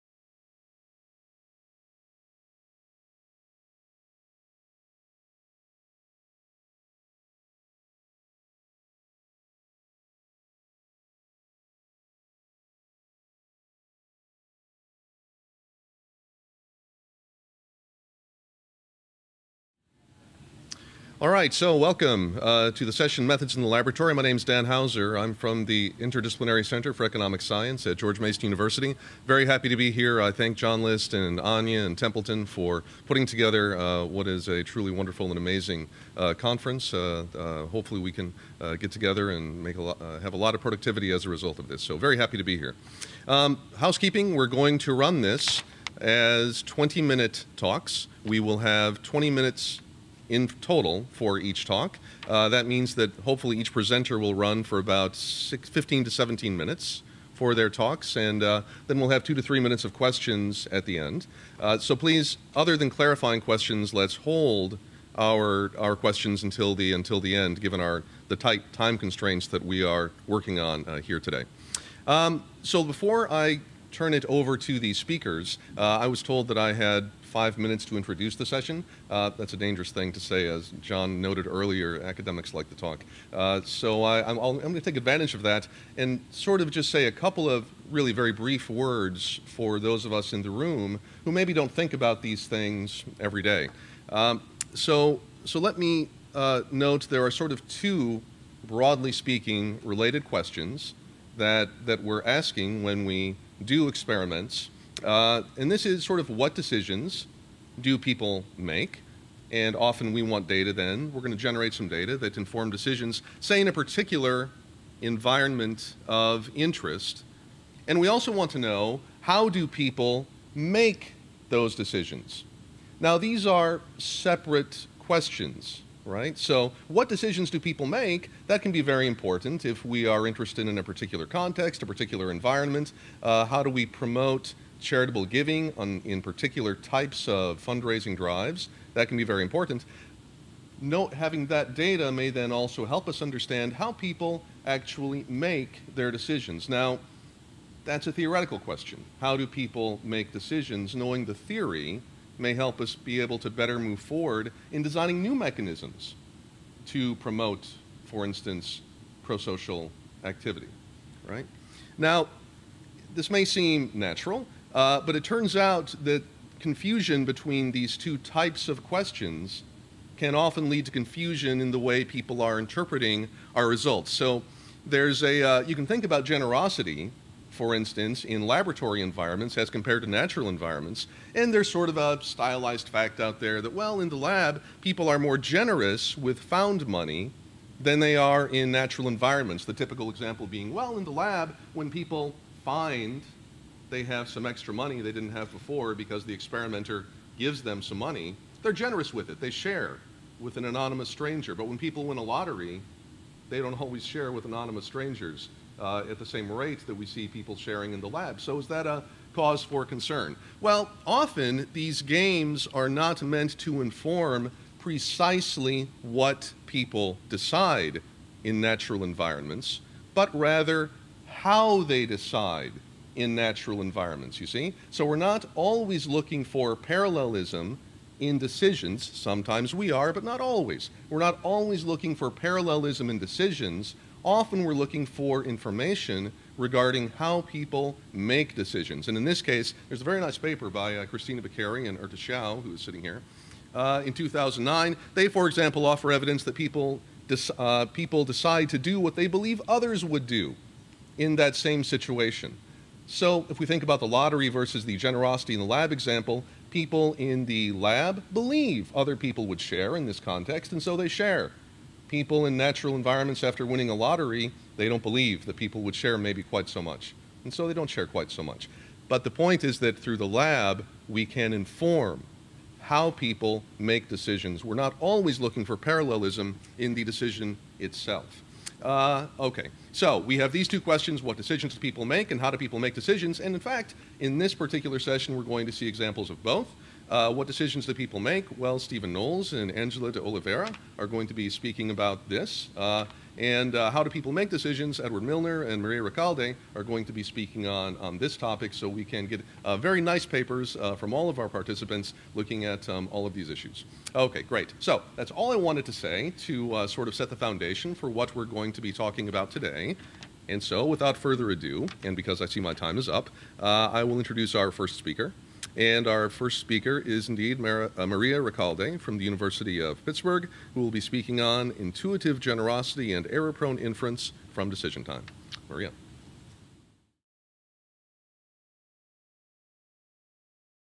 Session Introduction - Methods in the Laboratory - Dan Houser
Methods in the Laboratory (Introduction)
 - Dan Houser, George Mason

Biography: Dan Houser is Professor and Chairman in the Department of Economics at George Mason University. Dan is also the Director of ICES, the Interdisciplinary Center for Economic Science. Dan's recent research projects in the area of philanthropic giving have included experiments in public goods contributions, fairness, and behavior in social dilemmas.

Event & Venue: 2013 SPI Annual Conference, University of Chicago.
Session: Methods in the laboratory
Date: Oct 18th - 19th 2013

Dan Houser, Methods in the laboratory, 2013 SPI Conference, SPIhub.org, Science of Philanthropy Initiative